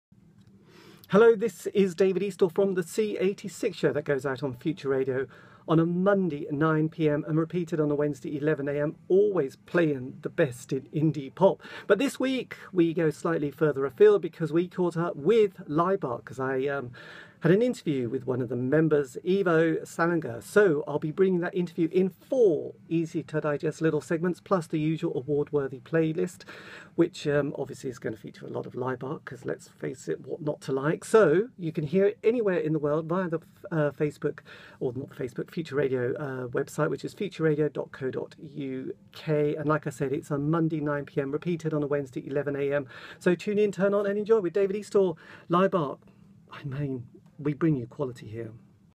C86 Show 13th & 15th November Laibach special
The C86 Show goes out 9pm (GMT) Monday & 11am (GMT) Wednesday - this week's special guests are Laibach because I caught up with Ivo Saliger to find out more about life in a Slovenia band, plus their new album, tour & much much more.